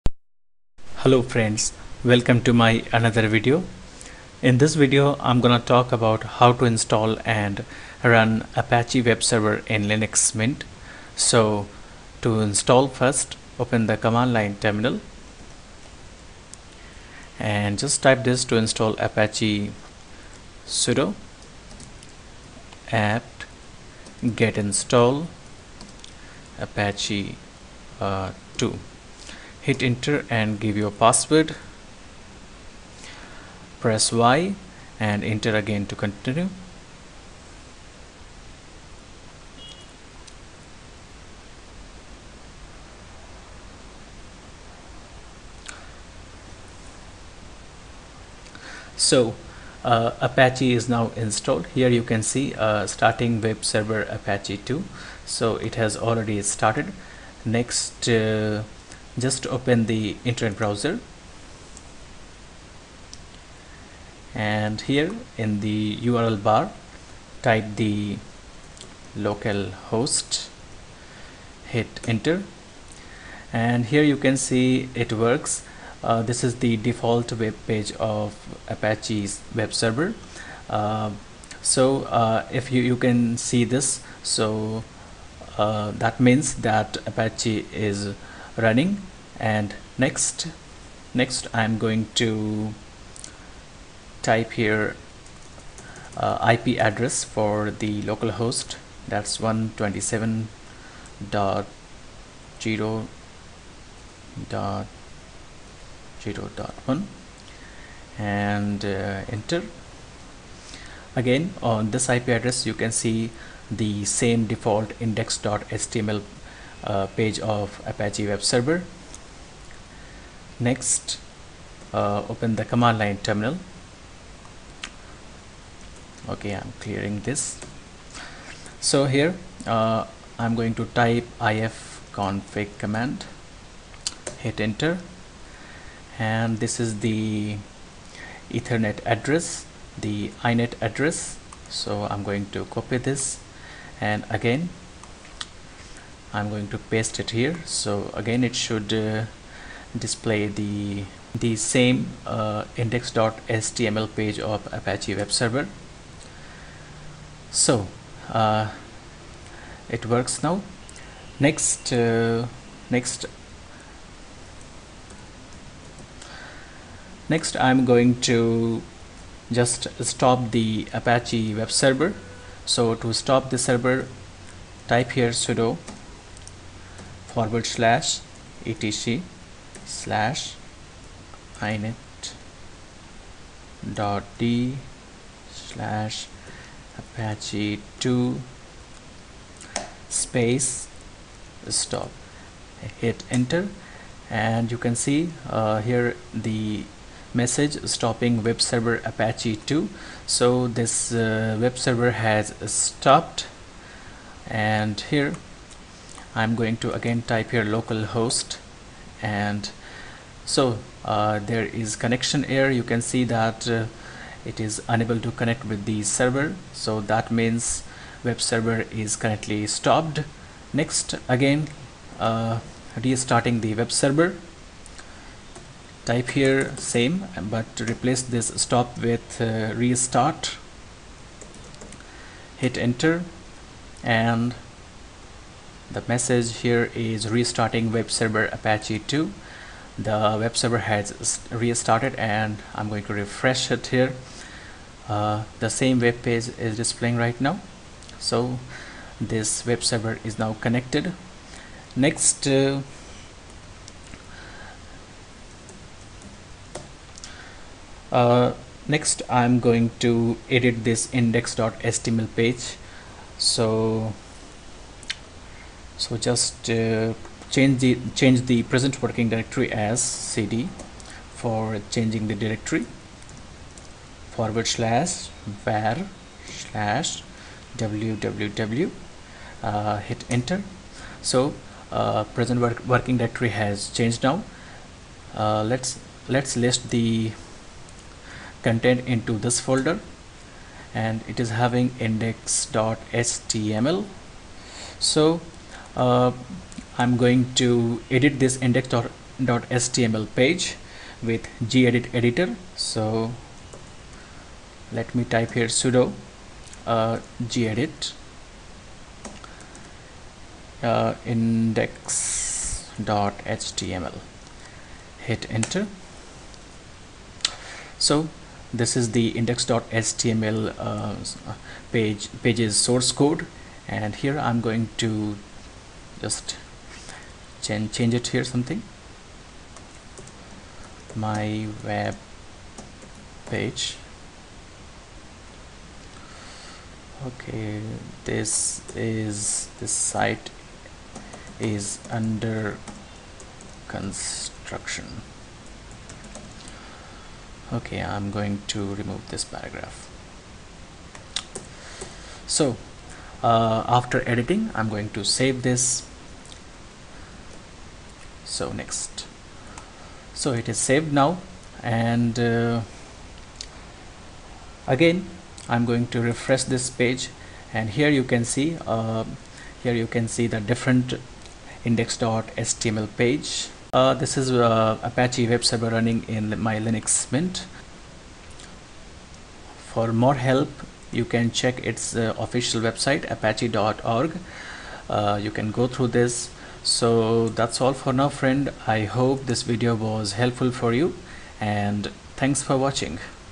Install & Run Apache Web Server in Linux Mint ( Ubuntu)
Command to Install Apache :
sudo apt-get install apache2

𝗛𝗼𝘀𝘁 𝘆𝗼𝘂𝗿 𝘄𝗲𝗯𝘀𝗶𝘁𝗲 𝗼𝗻 𝗗𝗶𝗴𝗶𝘁𝗮𝗹𝗢𝗰𝗲𝗮𝗻 𝗖𝗹𝗼𝘂𝗱 𝗦𝗲𝗿𝘃𝗲𝗿. 𝗚𝗲𝘁 $𝟭𝟬𝟬 𝗰𝗿𝗲𝗱𝗶𝘁 𝗳𝗼𝗿 𝟲𝟬 𝗱𝗮𝘆𝘀.

The Apache Software Foundation's goal is to build a secure, efficient and extensible HTTP server as standards-compliant open source software. The result has long been the number one web server on the Internet.